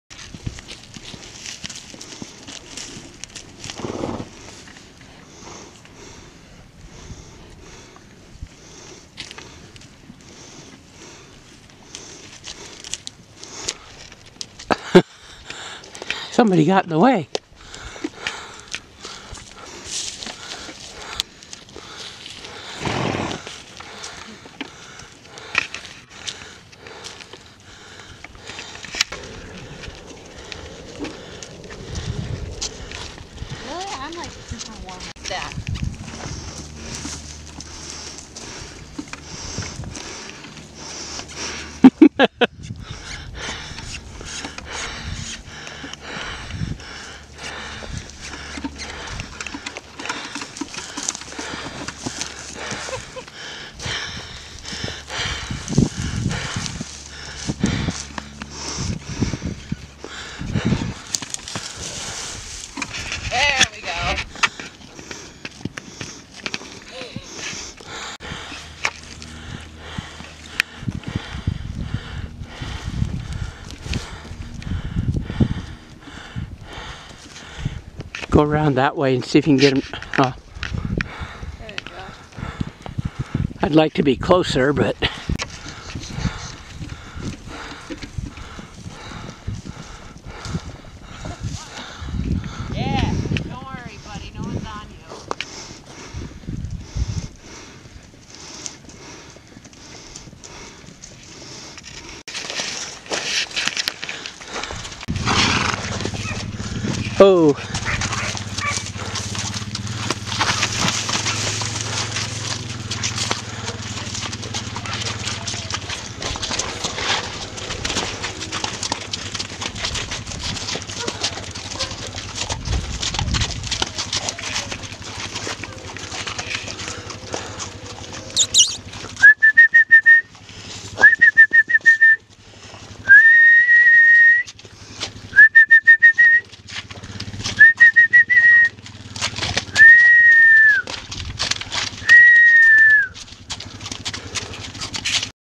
DunnyEclipseMoving
Mules moving Dunny and Eclipse